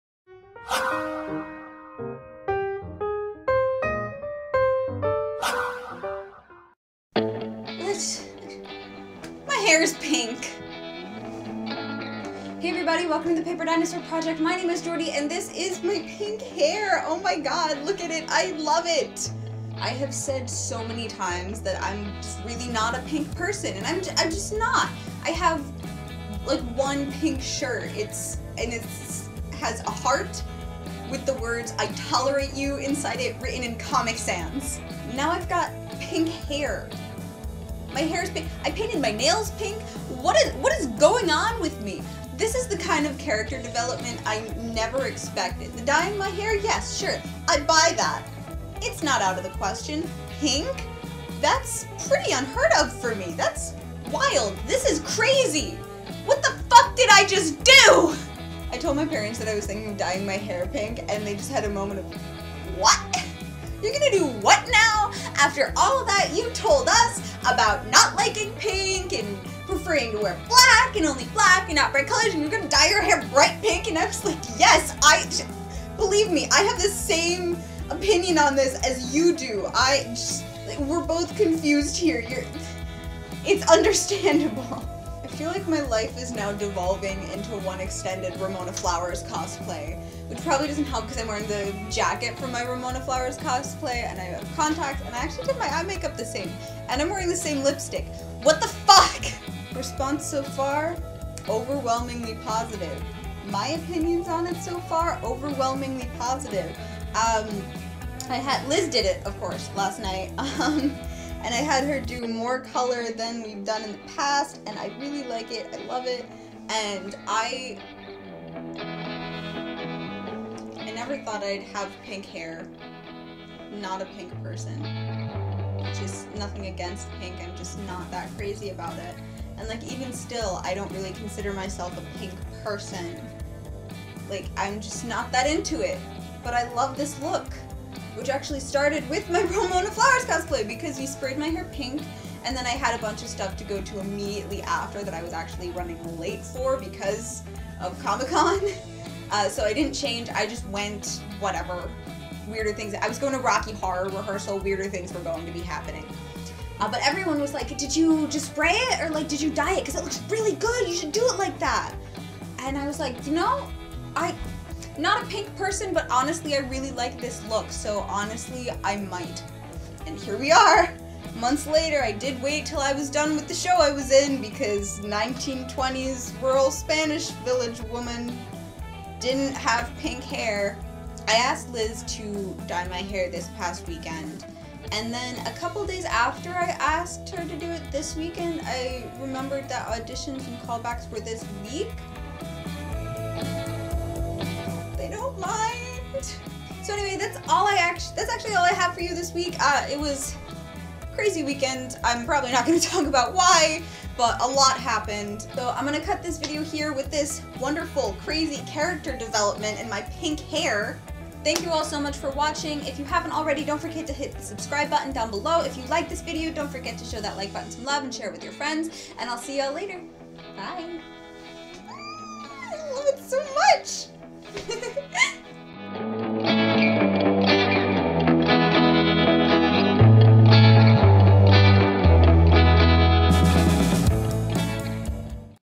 Make It PINK
I dyed my hair pink!  Your eyes aren't deceiving you, this is an actual real life cannon thing that happened to me personally.  I know, I'm still in shock too.